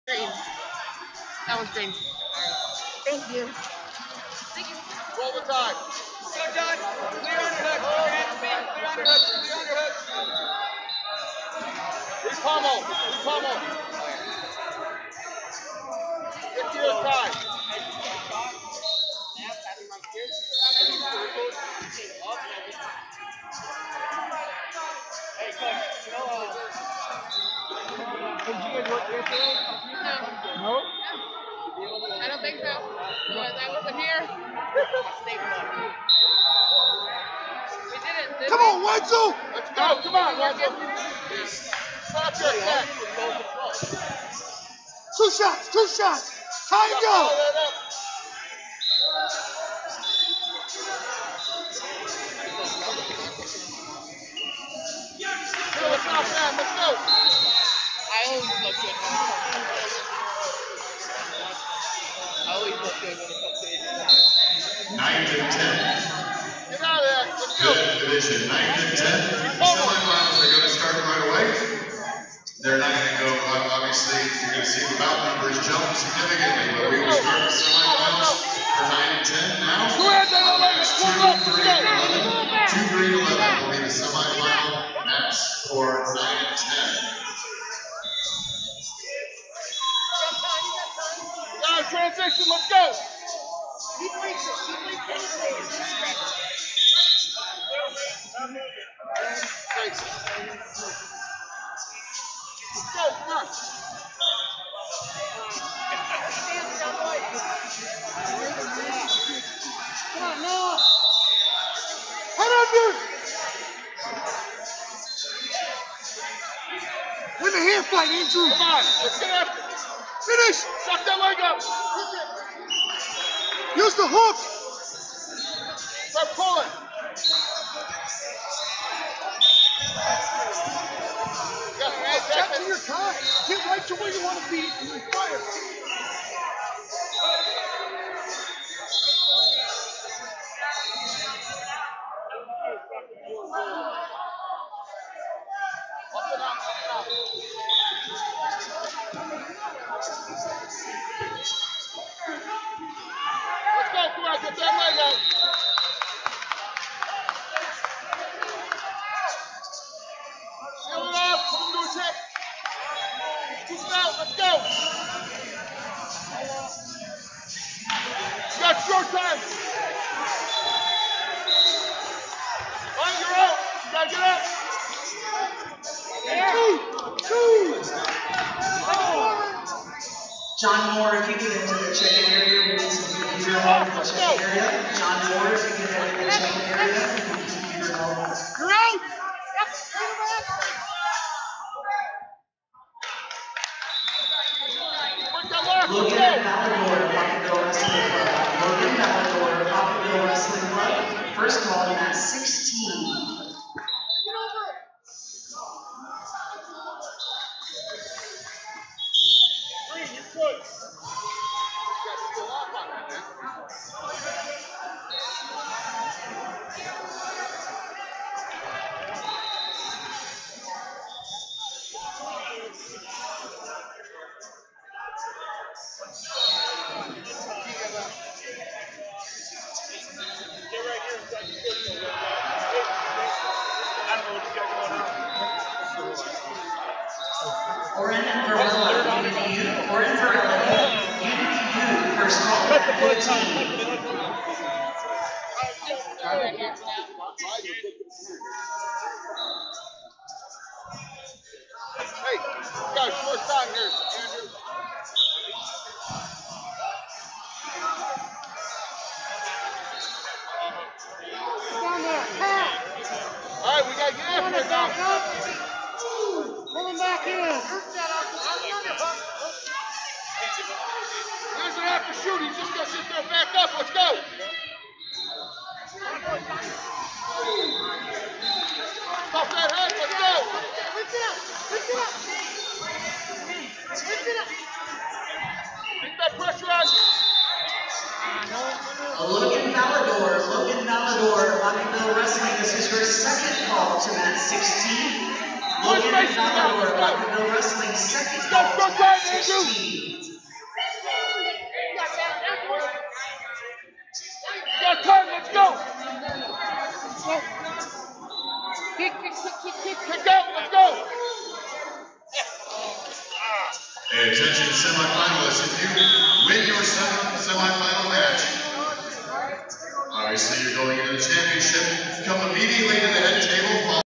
2020 USAW Preseason Nationals/2007 High School 11th-12th Grade 182 Andrew Wenzel Izzy Style Wrestl
2020 USAW Preseason Nationals - 2020 USAW Preseason Nationals/2007 High School 11th-12th Grade 182 Andrew Wenzel Izzy Style Wrestling Vs John Gunderson Baraboo Wrestling Club